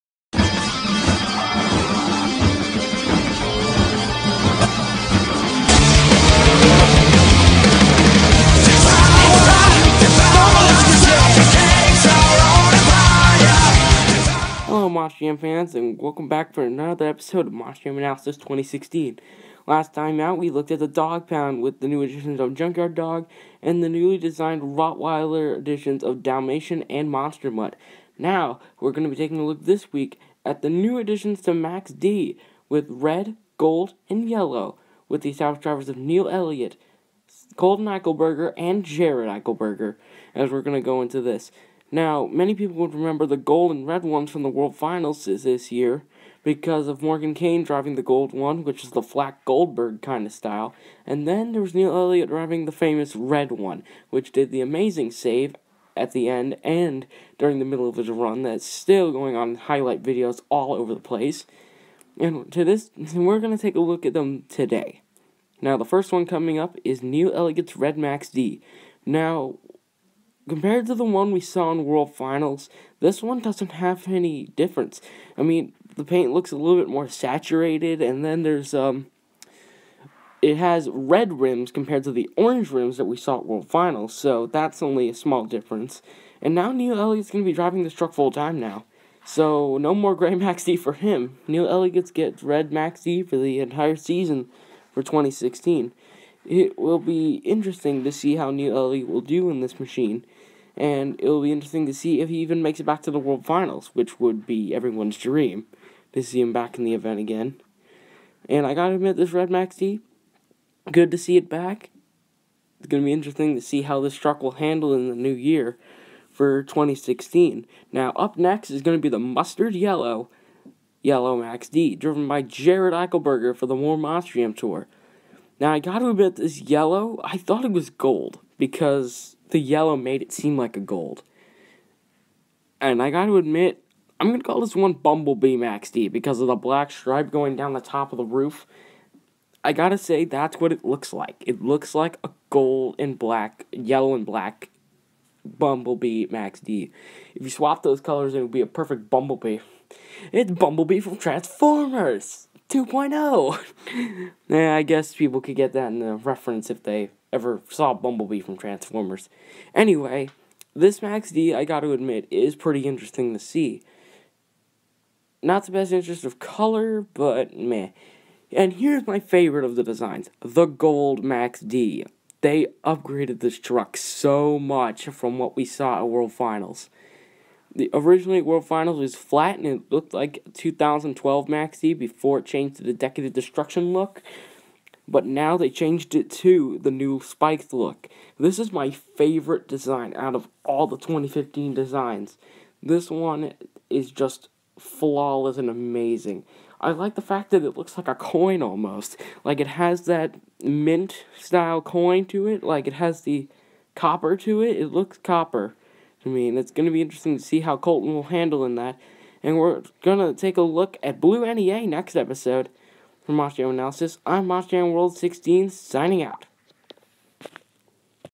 Monster Jam Analysis: Team Max-D
Today we take a look at the red, gold, and yellow Max-D's for the 2016 season! Next episode will be on Blue NEA.